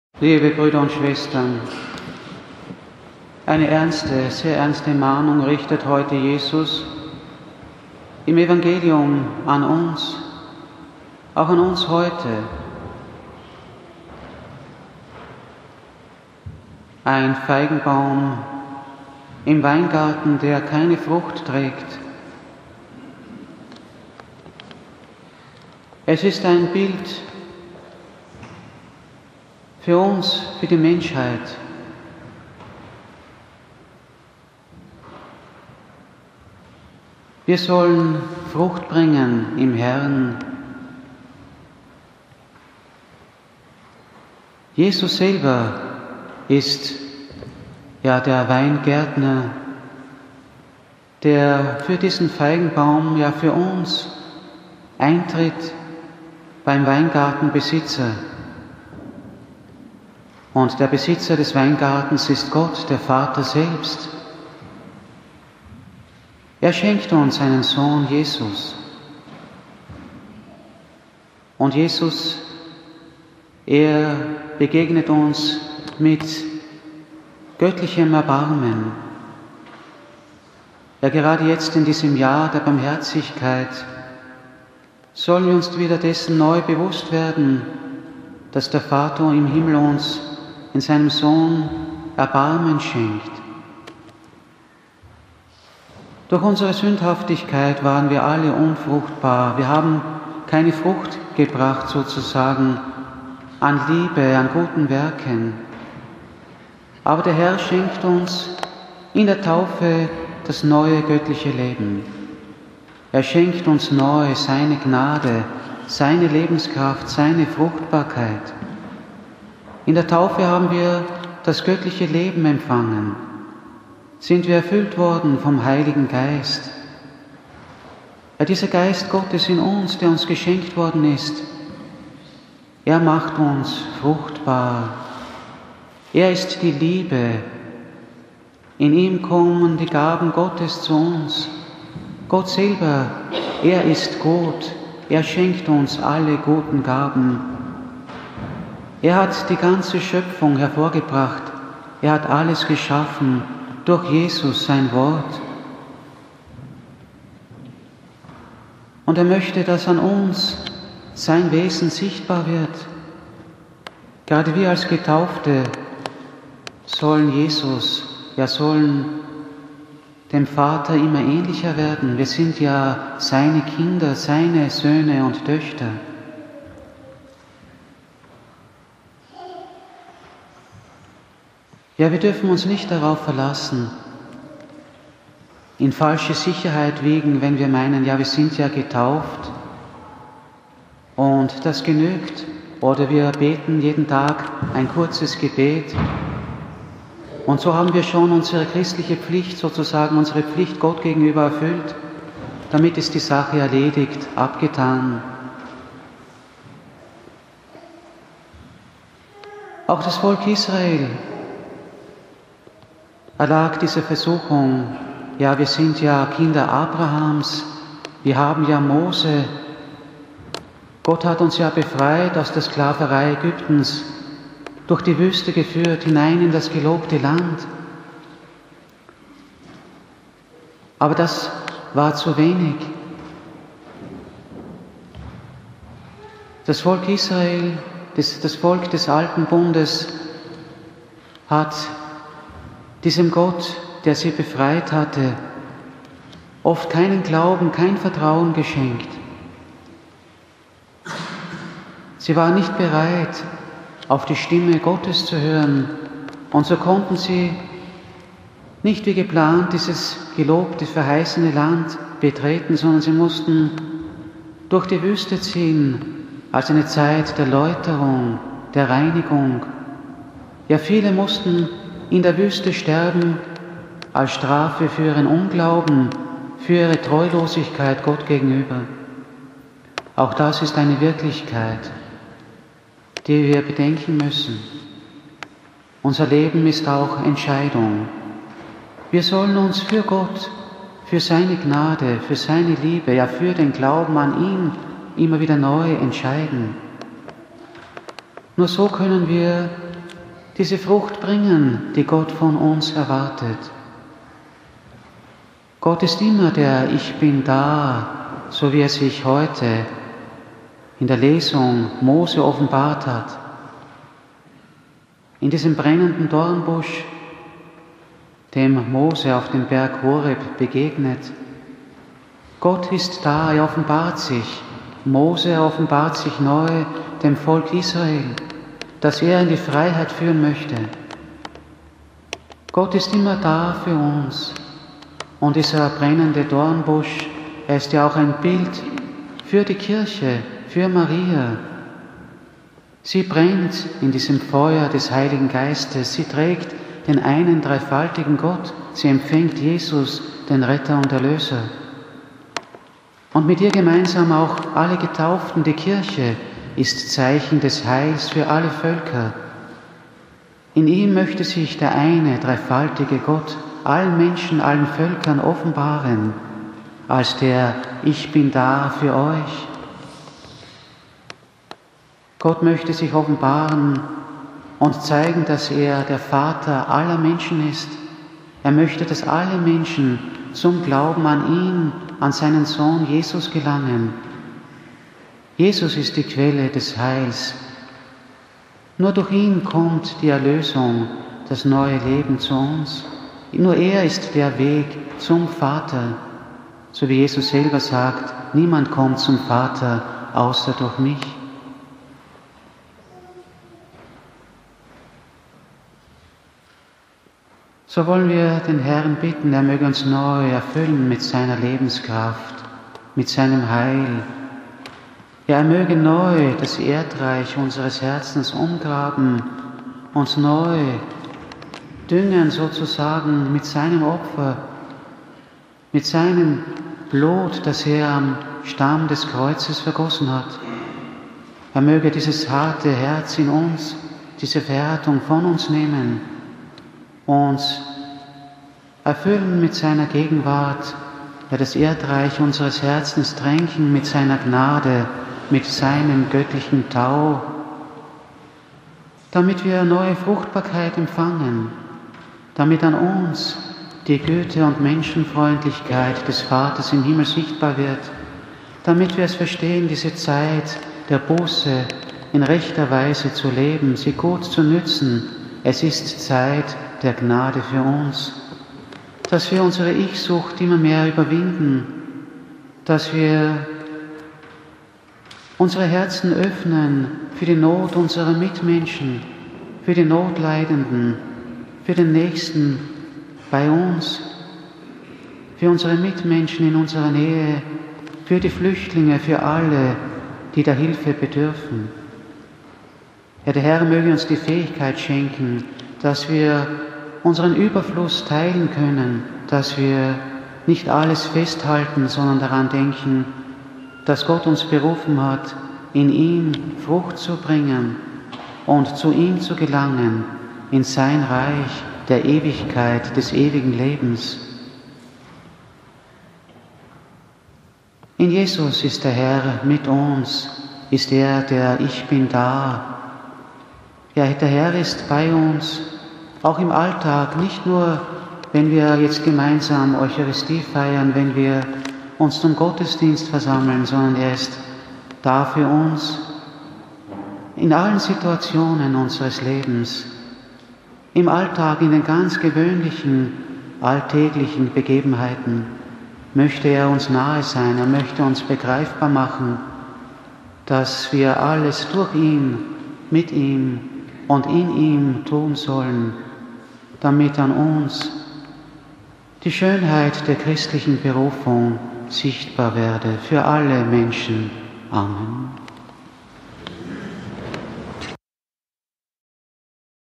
3. Fastensonntag C - Helmut Glieder OFM (2016)
Eine Predigt zum 3. Fastensonntag C
von P. Helmut Glieder OFM
2016

ERSTE LESUNG Ex 3,1–8a.13–15
PSALM Ps 103,1–2.3–4.6–7.8 u. 11
ZWEITE LESUNG 1 Kor 10,1–6.10–12
EVANGELIUM Lk 13,1–9